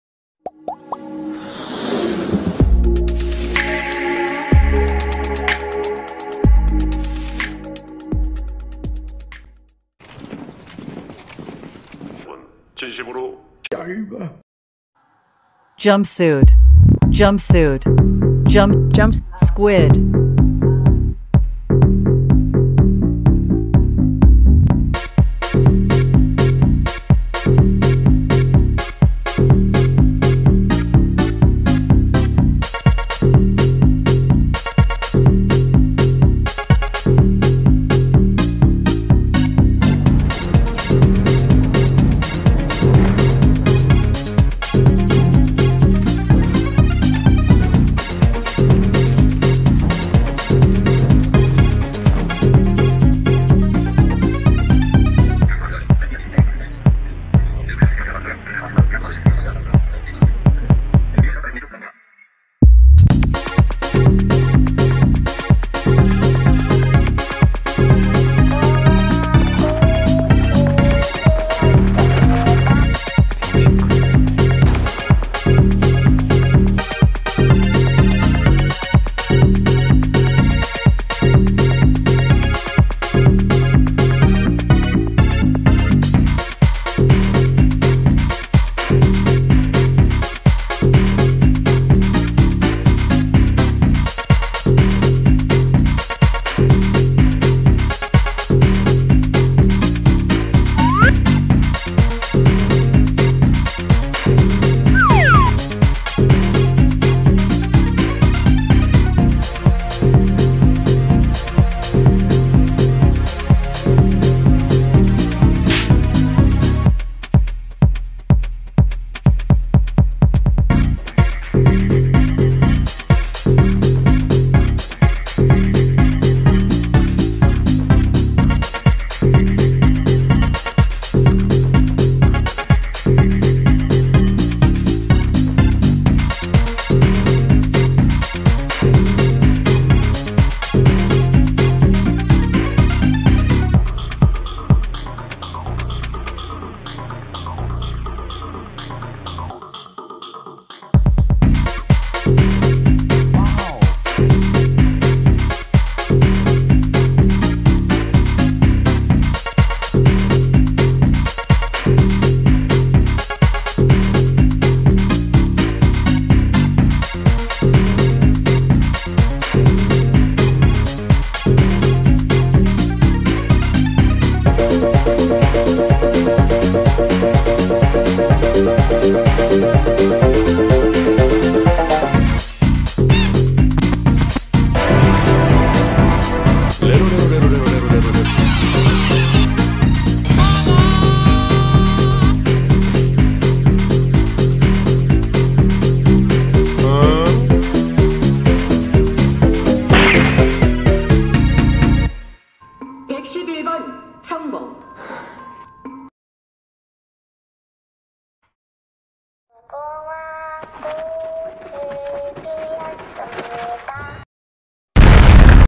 CLO-3D 경비병 얼굴 동그라미 마스크/얼굴에 동그라미 그려넣는 방법 (MV)
작업데이타중에 오징어게임 경비병이 입은 점프슈트와 비슷한게 있어서
 CLO-3D로 아바타에 입혀 보았습니다
오징어 경비대 MASK 대신 얼굴에 Texture 색을 적용하여
오징어경비병 특징인 마스크를 구현해 보았습니다.

헤드폰이나 이어폰을 사용하세요.🎧🎧🎧
좀 더 감동적인 영상이 될 겁니다.🧡
 Use headphones or earphones.🎧🎧🎧
It's going to be a more emotional video.🧡